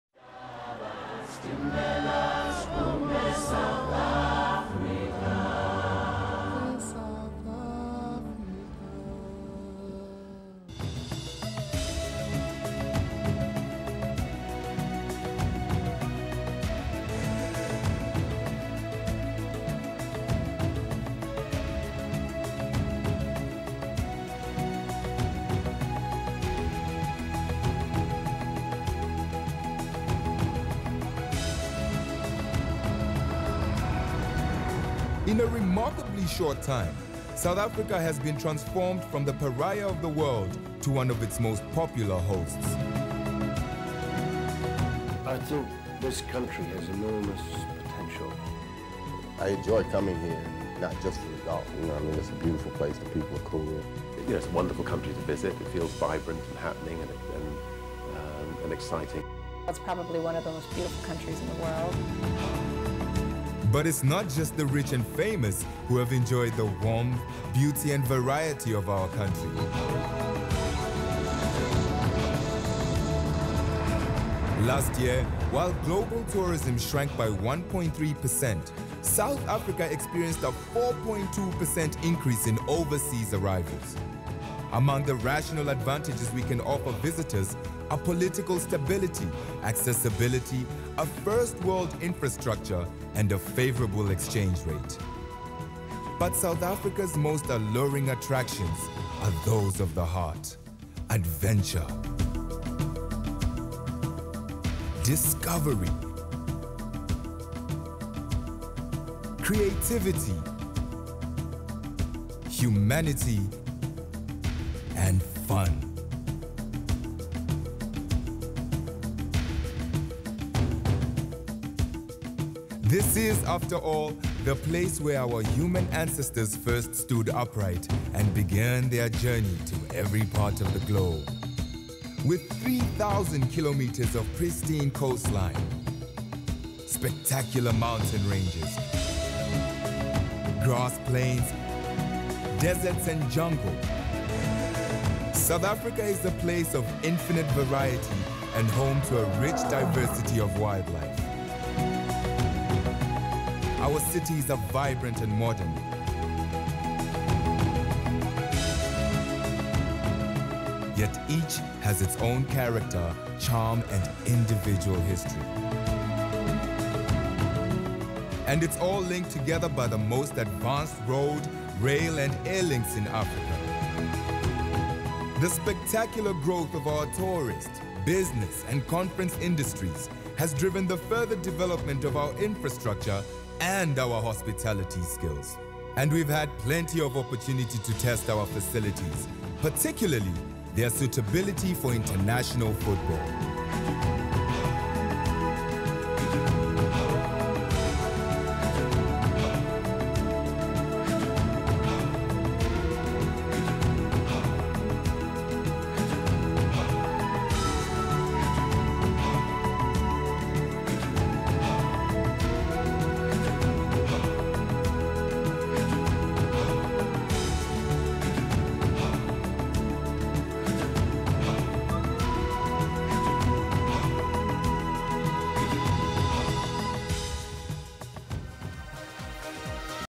2010 Soccer World Cup Bid AV
In May 2004 The Bridge was commissioned to edit all the audio visual material for the SA delegation's presentation to FIFA. This is one of the clips.